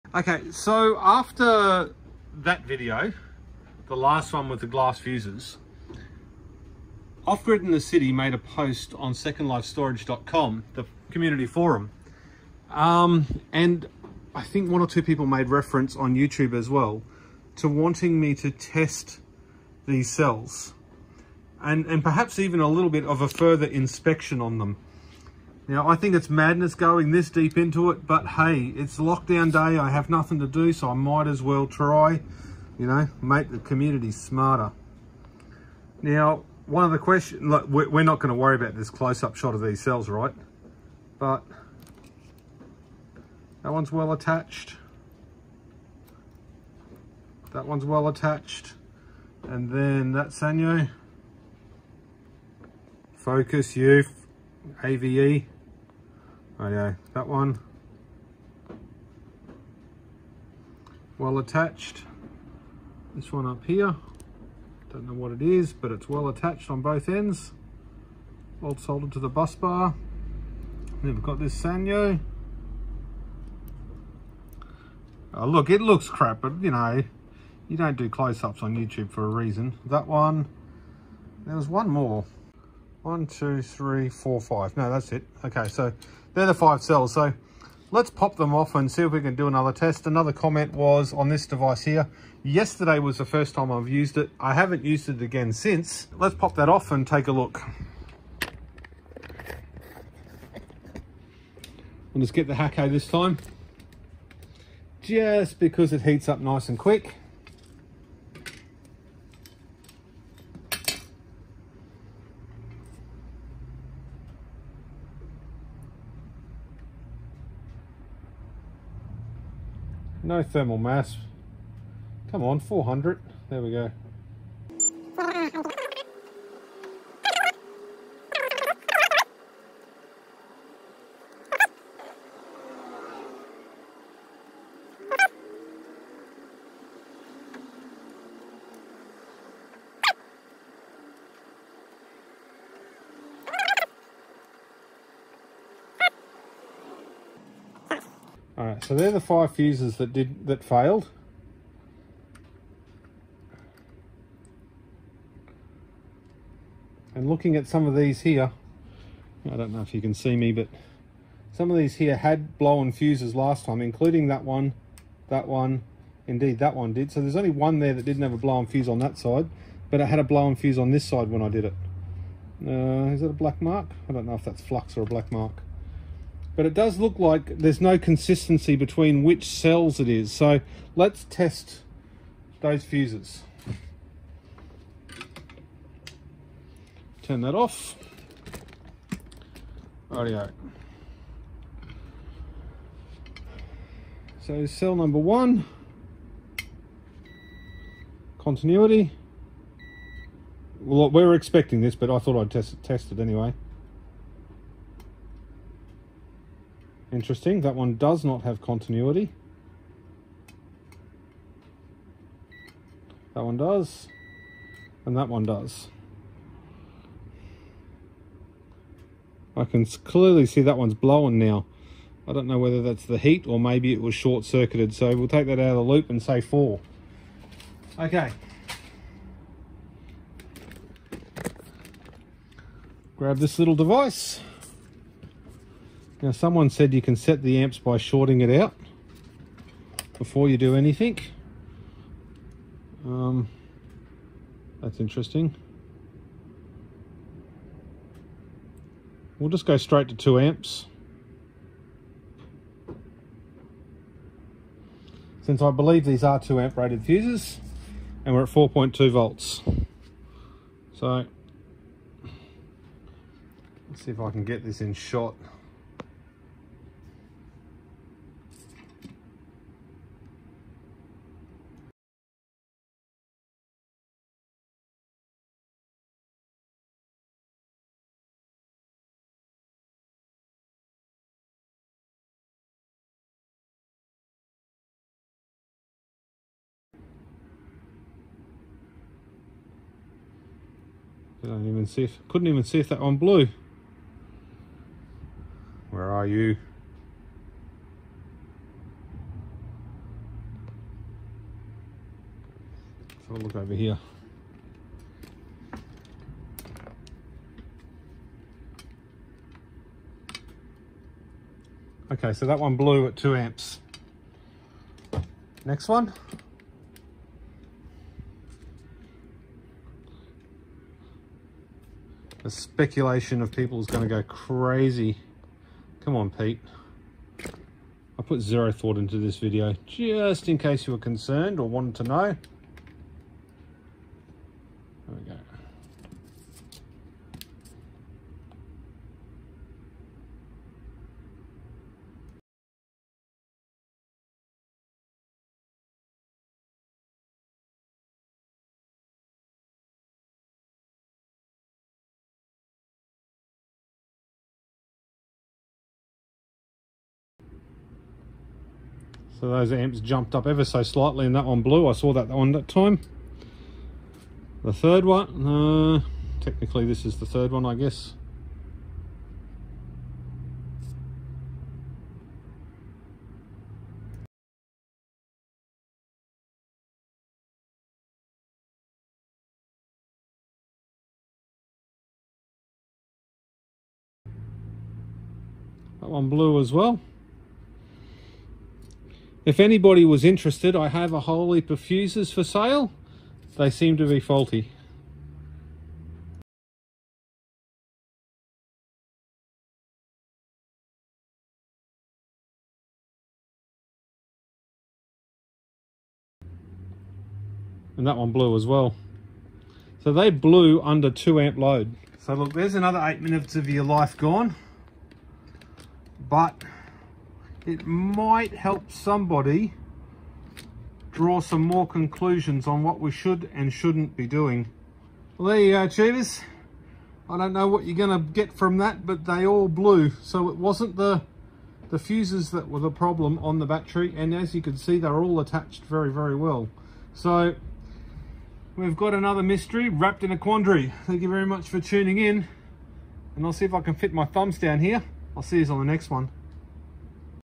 Testing the 5 fuses from yesterdays video
I decided to pull those five fuses and see if they would blow at 2 amps - they did as it turns out so it wasn't bad fuses that caused the issue. Neither was it the cells as all five cells had previously blown the wire fuses.

🔵 Glas Fuses
🔵 Soldering with thermal mass